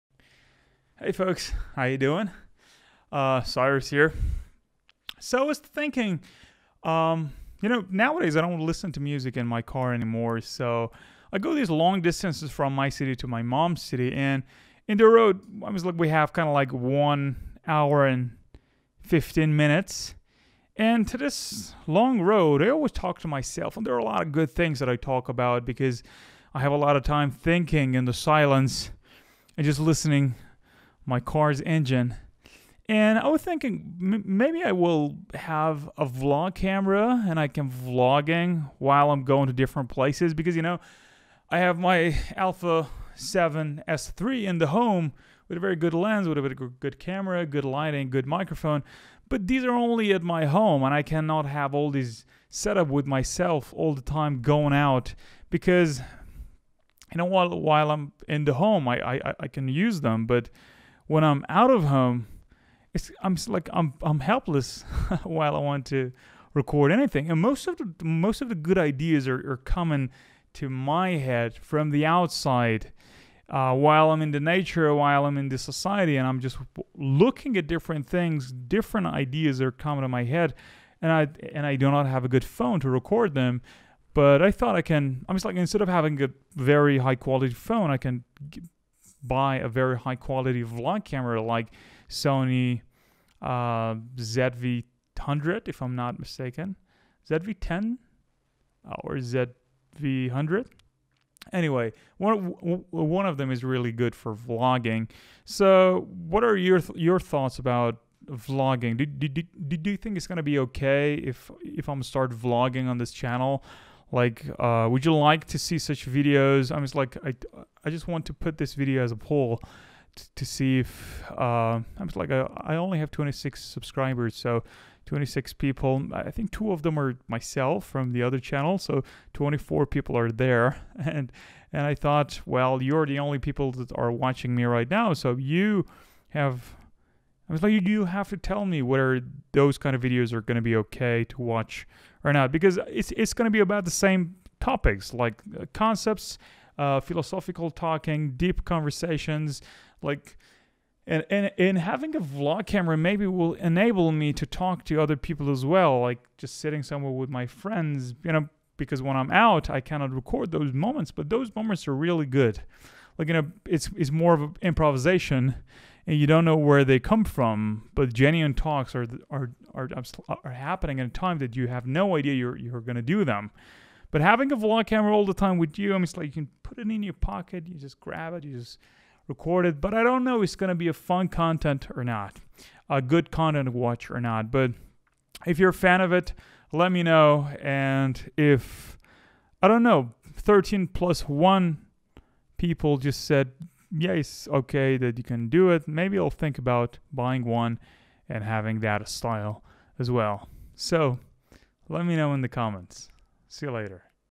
Vlogging Style | Yes Or No ????
Well I have 2 or 3 videos which It's more or less a vlog, back in the time when I was in Oman. I really like it myself, as a matter of fact I didn't want to go for such a thing, as I mentioned that earlier once, but I wanna mix that with what I have right now, cause there are a lot of good talks happening when I'm all alone talking only for myself :D
Just wanted to know your opinion, There are no rushes, Leave your comments when you see this video.
Thanks In advance for your honest opinion.
Cyrus